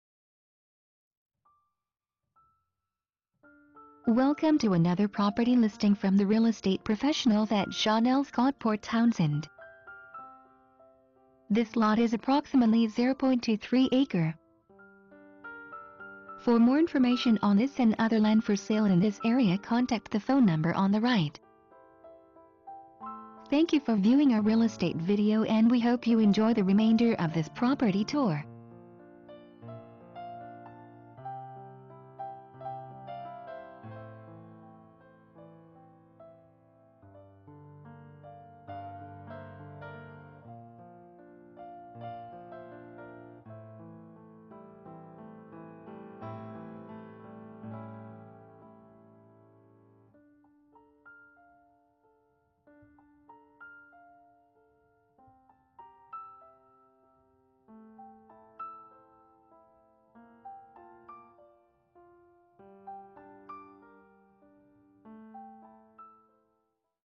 Land For Sale Port Townsend WA 0.23-Acres $90,000 Port Townsend WA
Listing Address: 7 and  Grant   Port Townsend, WA  98368
Property Type: Land
Price: 90,000
Agent Name: Dale Barron

Agency: John L Scott Port Townsend

Description: Double water view lot close to hospital and shopping. Sunny location with great soil. Presently used as community garden.